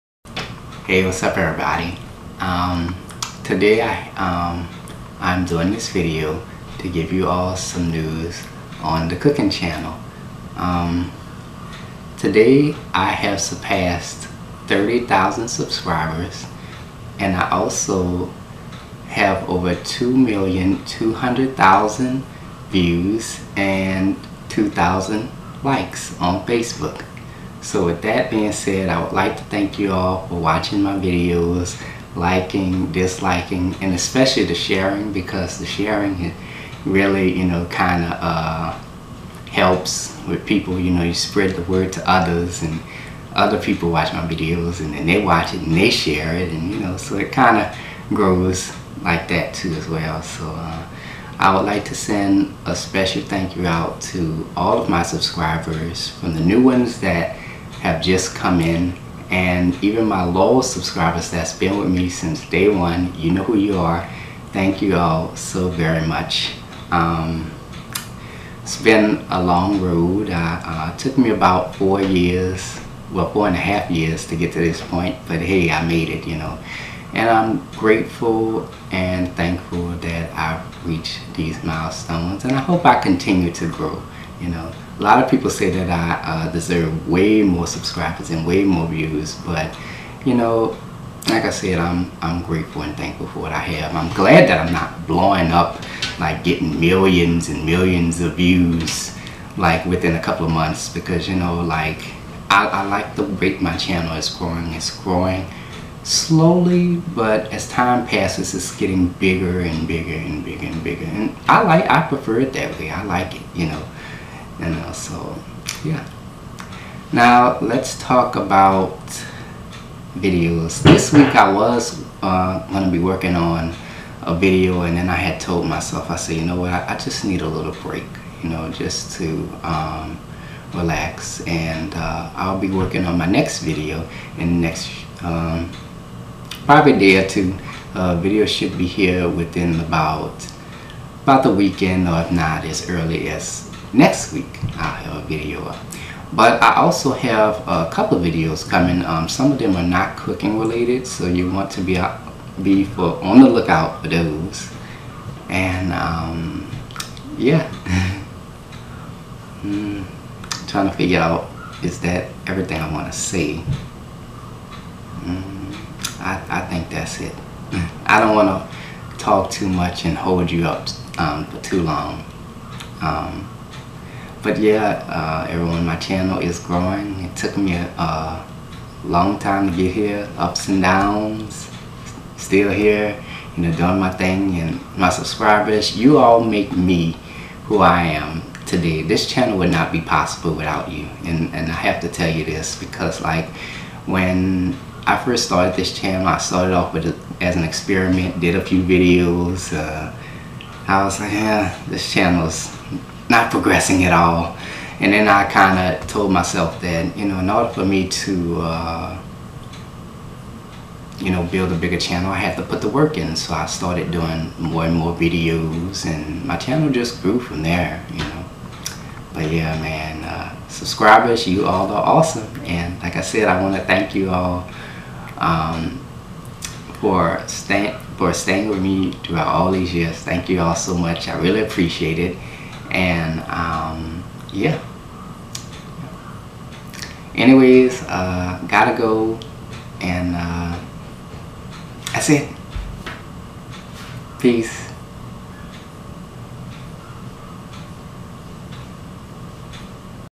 Charlie reaches 30,000 subscribers. Thank you all!
Charlie Andrews has reached 30,000 subscribers, 2, 200,000 views and 2 thousand likes on Facebook. Thank you all so much! This channel would not be possible without you!

$CharlieA504